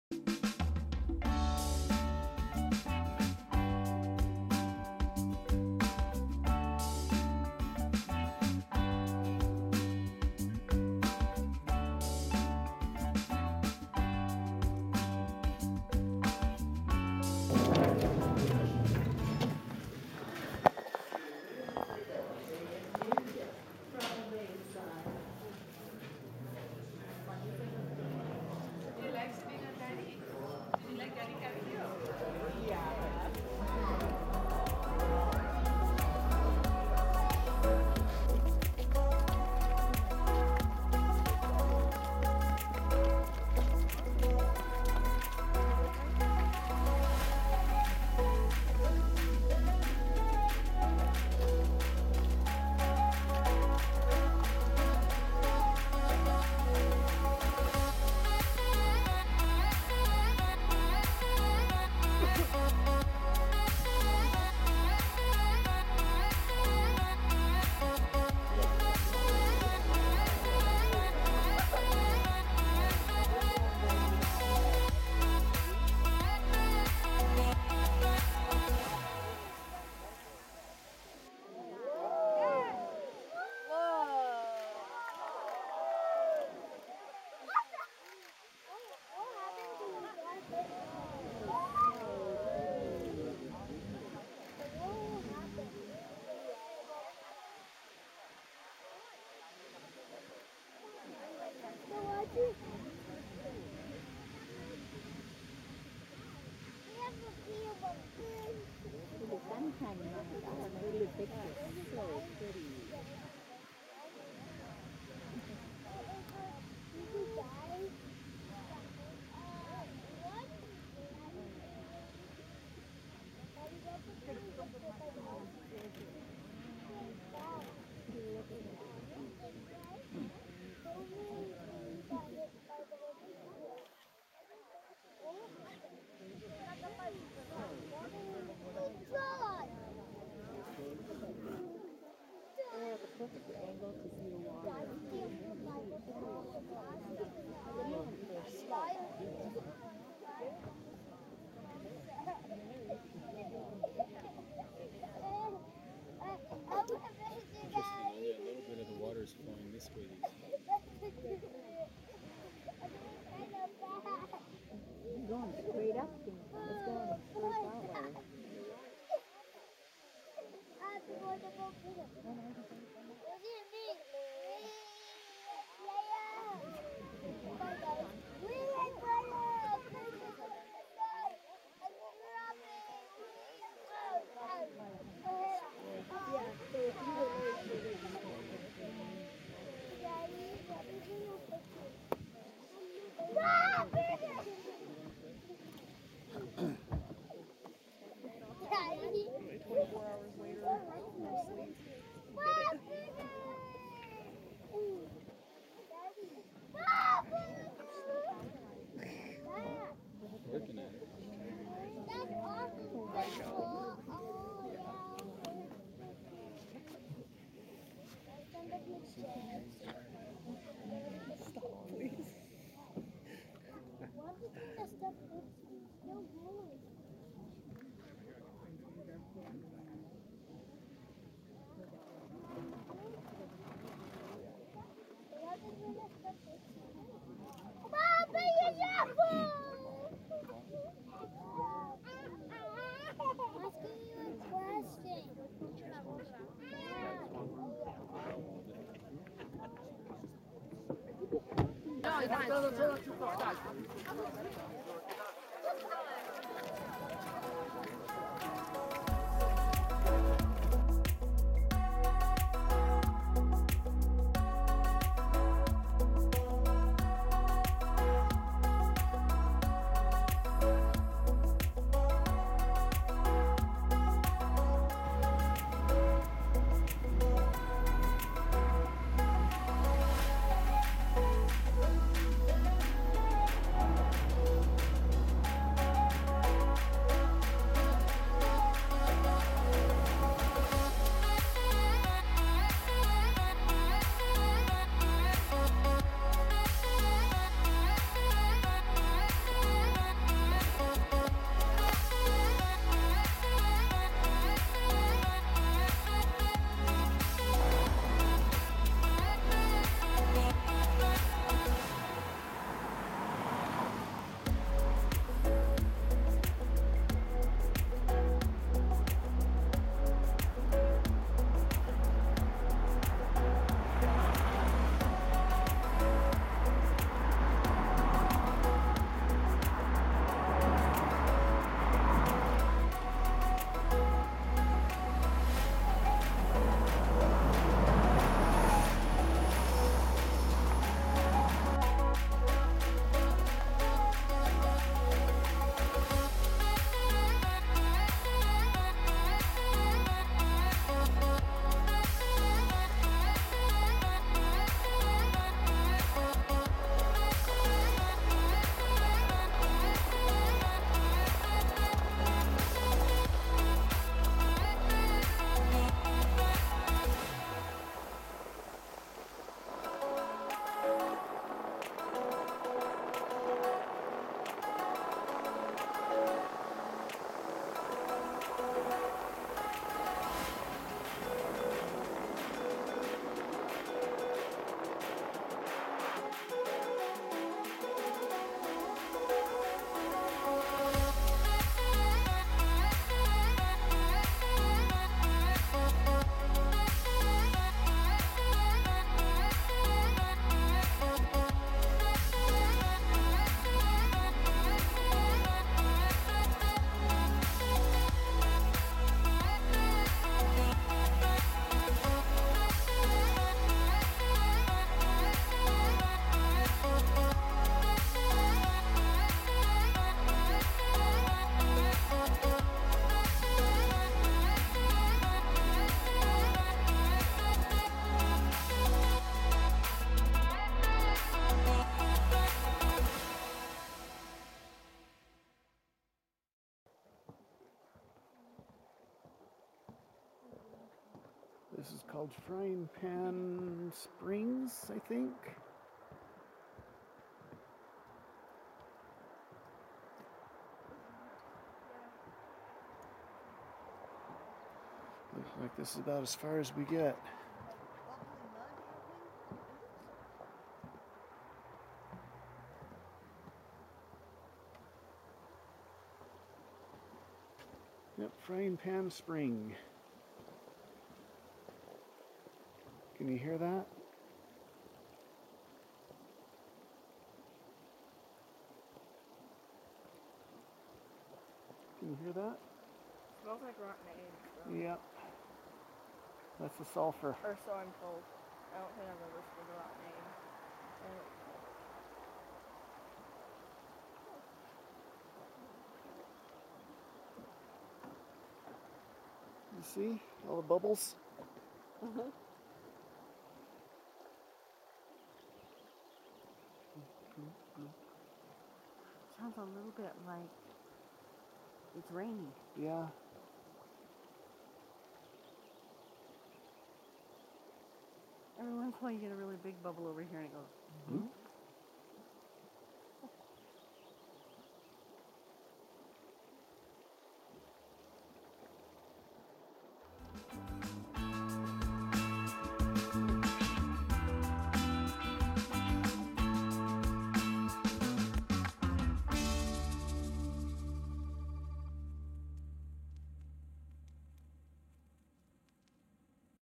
Indifferent Bison and a Brave Raven - a 360 VLog
Our second day in Yellowstone National Park. Old Faithful, Bison, and a raven.